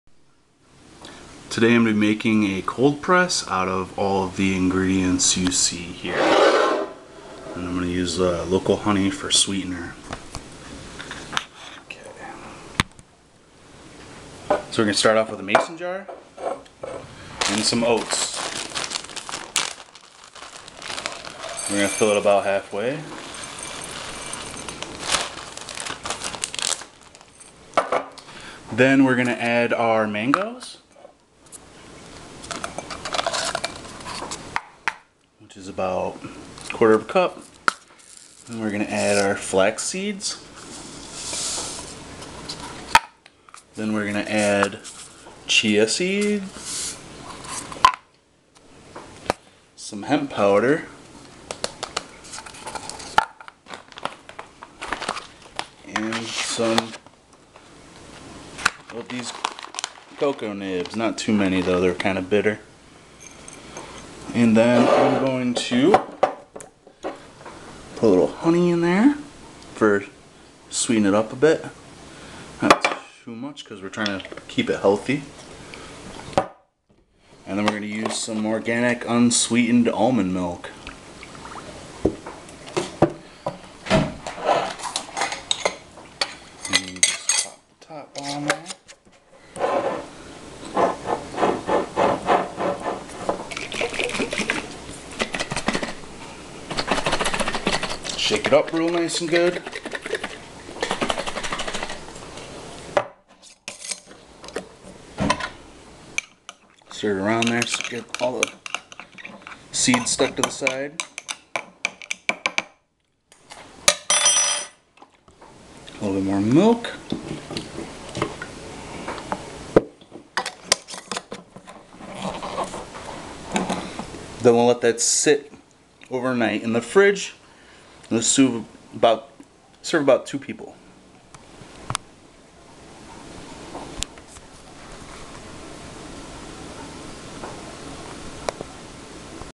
Cold Press Oats Cereal
This is a simple demonstration on how easy it is to get a well rounded start to your day with a breakfast packed with nutrient rich super foods. In this video I made a cold press oat cereal using;
1 cup of Organic Oats,
1/4 cup of Organic Dried Mango cubes,
1/8 of a cup Organic Flax Seeds,
1/8 of a cup Organic Chia Seeds.
1 small handful of Organic Cocoa Nibs,
2 tbsp of Local Organic Honey,
1 cup of Organic Almond
I combine all the dry ingredients then pour the milk and honey over mix well then add more milk after words depending on room, I let it sit over night and its the exact texture I want for a cold oatmeal cereal. about two servings very filling. Enjoy!